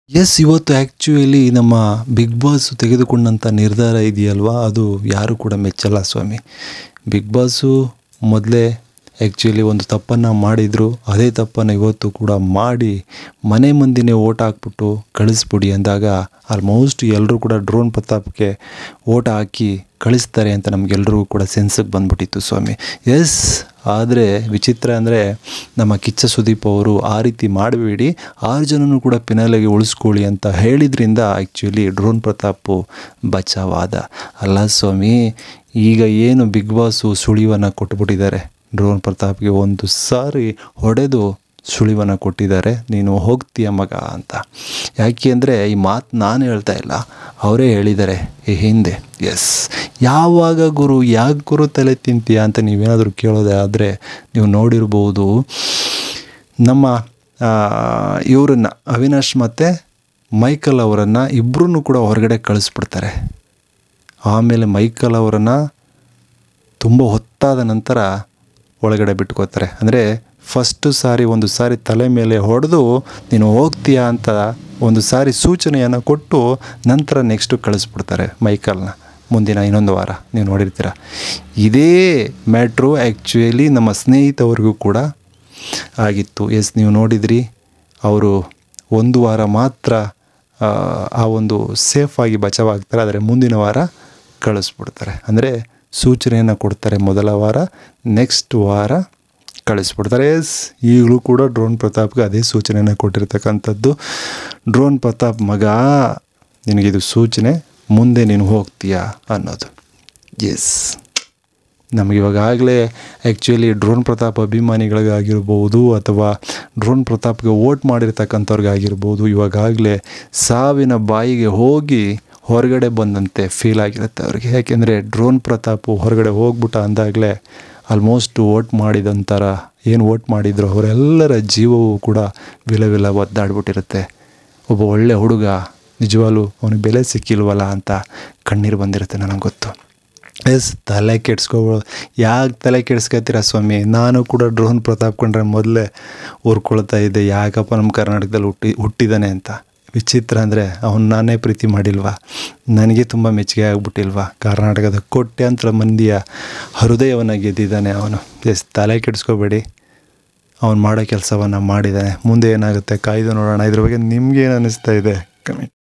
ಡ್ರೋನ್ ಪ್ರತಾಪ್ ನ ಆಚೆ ಕಳಿಸುವ ಸೂಚನೆ ಕೊಟ್ಟ ಬಿಗ್ ಬಾಸ್.! ಇದೇನು ಸತ್ಯತೆ | Bigboss 10 Kannada | Drone pratap
Bigboss 10 Kannada